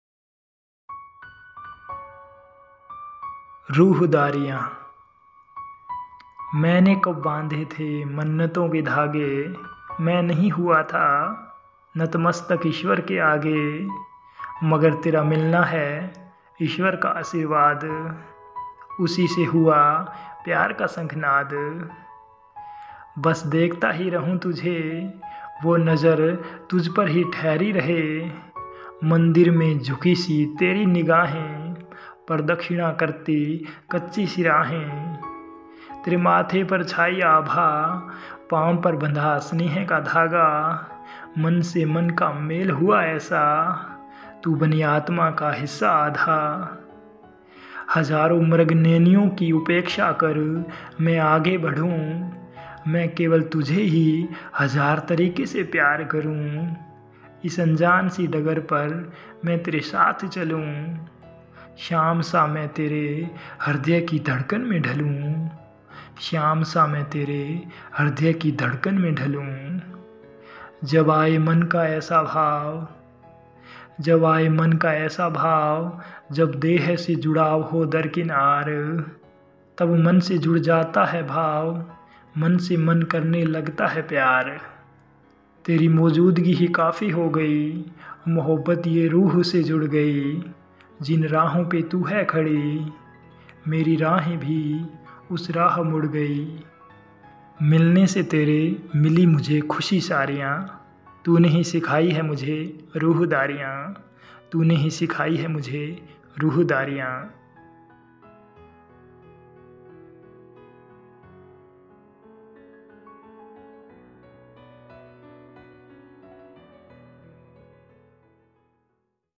स्नेह आलाप -57 ❤️【रुहदारियां】anand sharma hindi poetry | love poem
Written by  : anand sharma
Vocal :  anand sharma

स्नेह आलाप : मन के भावों और बोध से उकेरा गया अद्वितीय ग्रंथ

 स्नेह आलाप - 57【रुहदारियां】

मैंने कब बांधे थे मन्नतों के धागे
मैं नहीं हुआ था नतमस्तक ईश्वर के आगे
मगर तेरा मिलना हैं ईश्र्वर का आशीर्वाद !
उसी से हुआ प्यार का शंखनाद ..

बस देखता ही रहूँ तुझे
वो नज़र तुझ पर ही ठहरी रहे ।
मंदिर में झुकी सी तेरी निगाहें !
प्रदक्षिणा करती कच्ची सी राहें ..

तेरे माथे पर छाई आभा,
पाँव पर बंधा स्नेह का धागा ।
मन से मन का मेल हुआ ऐसा !
तू बनी आत्मा का हिस्सा आधा ..

हजारों मृगनयनीयों की उपेक्षा कर, मैं आगे बढु ।
मैं केवल तुझे ही हजार तरीके से, प्यार करुँ ।
इस अनजान सी डगर पे,  मैं तेरे साथ चलूँ !
शाम सा मैं तेरे ह्रदय की धड़कन में ढलु .

जब आए मन का ऐसा भाव
जब देह से जुड़ाव हों दरकिनार
तब मन से जुड़ा जाता है भाव !
मन से मन करने लगता हैं प्यार ..

तेरे मौजूदगी ही काफ़ी हो गई,
मोहब्बत ये रूह से जुड़ गई ।
जिन राहों पे तू हैं खड़ी !
मेरी राहे भी उस राह मुड़ गई ..

मिलने से तेरे, मिली मुझे ख़ुशी सारियां !
तूने ही सिखाई हैं मुझे रुहदारियां ..

"मन की नाव एक स्वर्ग जैसी हैं
जब तुम मन को पढ़ते हो तो,
इससे ऊर्जावान अनुभूति जीवन में होती हैं
और हम स्वभाविक रूप से
नया बेहतरीन दृष्टिकोण ढूंढते हैं "

【 आनंद शर्मा 】

● Read On Amazon Kindle ●